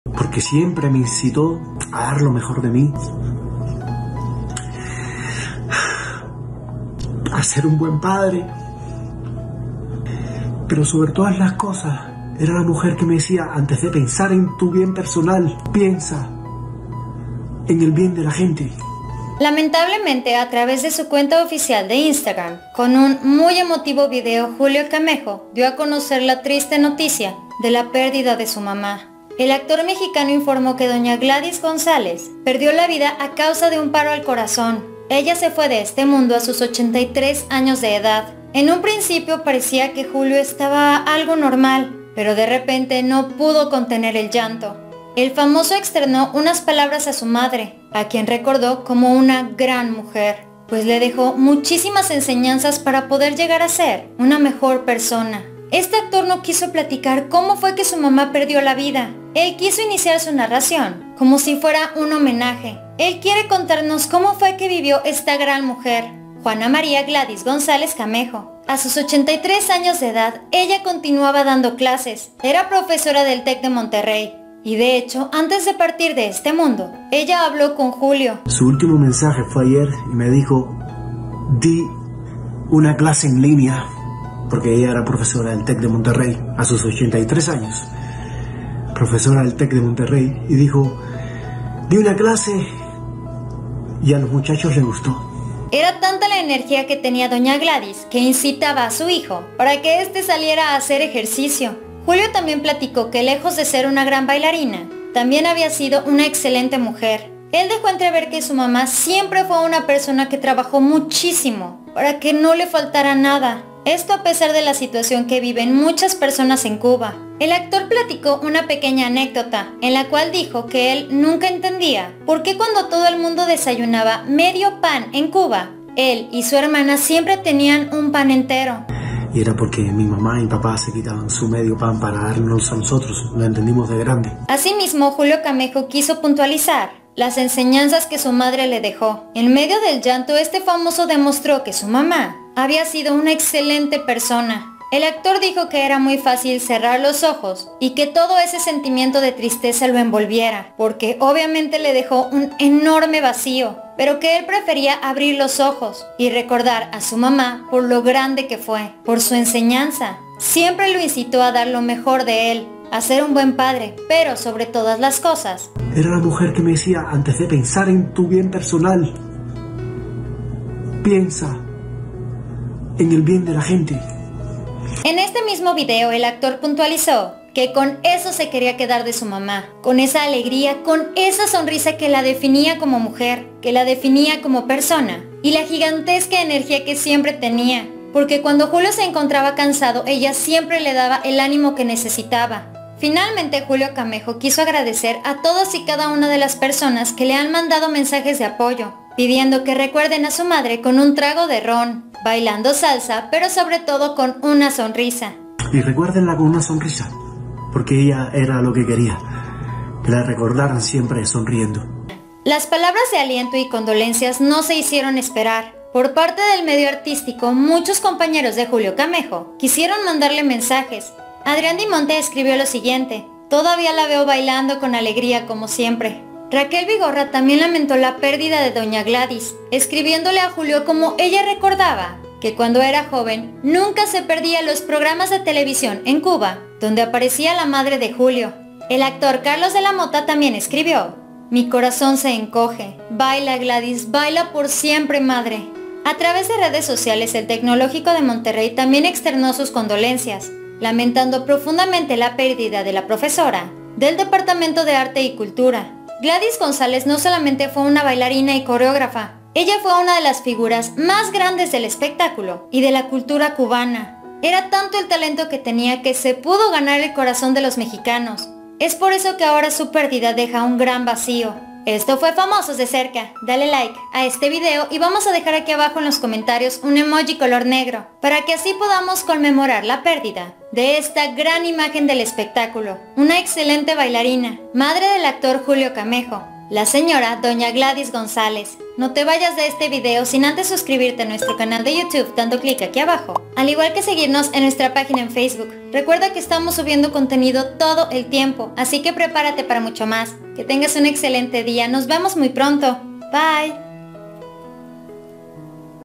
⛔😭 ¡ 𝗗𝗲𝘃𝗮𝘀𝘁𝗮𝗱𝗼 𝗝𝘂𝗹𝗶𝗼 𝗖𝗮𝗺𝗲𝗷𝗼 Ante la Partida de Su Madre ! ⛔🖤
Duro suceso que vive Julio Camejo, nuestras condolencias para el actor.

 BIENVENIDOS!!!